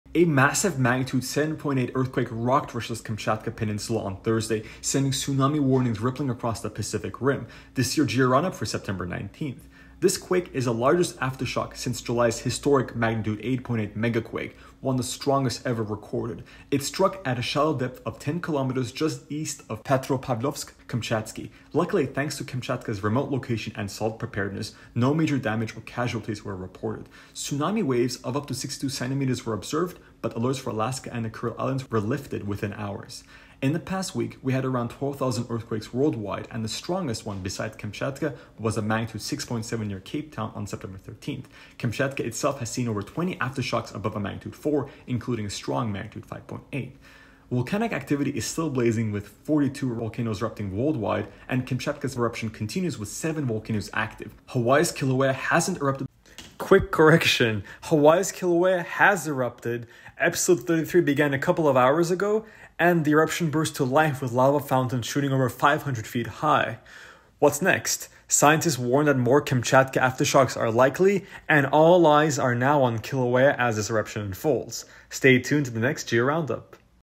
Earthquake and Volcano News: Geo Roundup September 19
Providing weekly news on earthquakes, volcanos and landslides. The Geo RoundUps are a bit sized taste of geology and geological engineering activity from around the world.